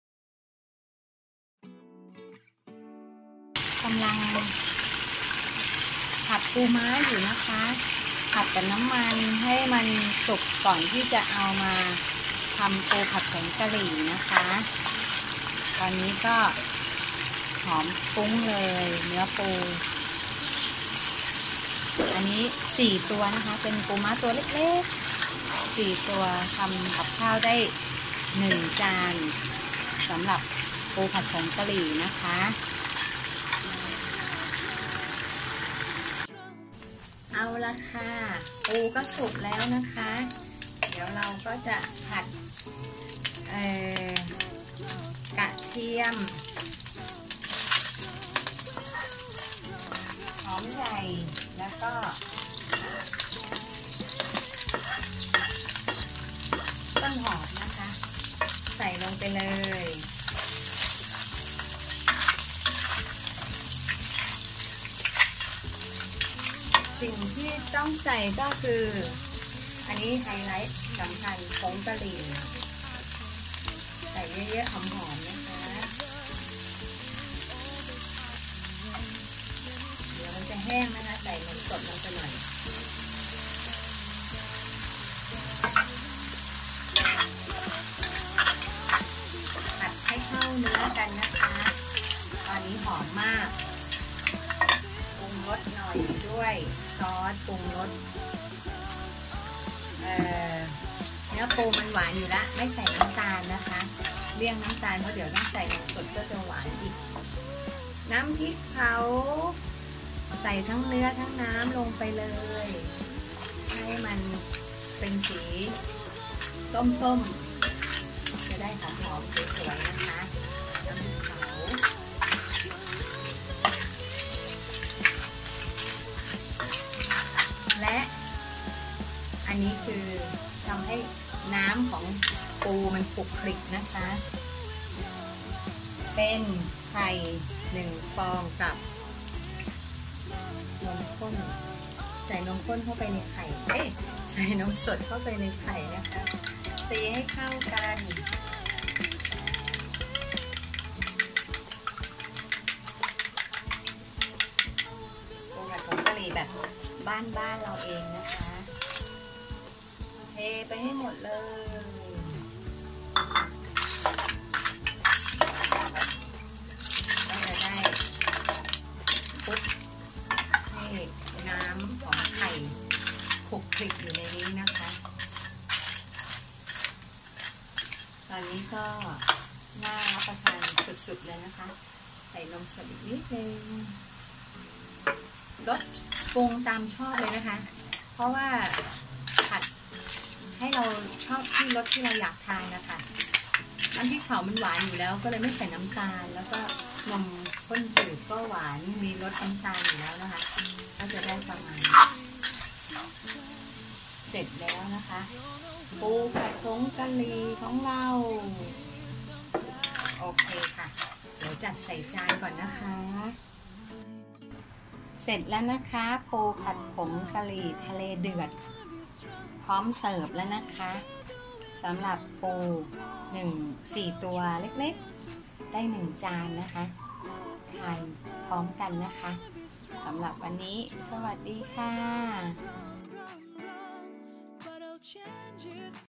ปูผัดผงกระหรี่ทะเลเดือด
เมนูระดับภัตตาคาร แต่ทำง่ายแสนง่ายแบบบ้านๆก็อร่อยได้จ้า

ด้วยวัตถุดิบที่มีอยู่แล้วในบ้าน
1.ปูทะเล
2.ต้นหอม, คึ่นช่าย, หอมหัวใหญ่
3.ผงกระหรี่
4.น้ำพริกเผา
5.ไข่ไก่
6.นมสดหรือนมข้นจืด

ลองทำดูนะคะ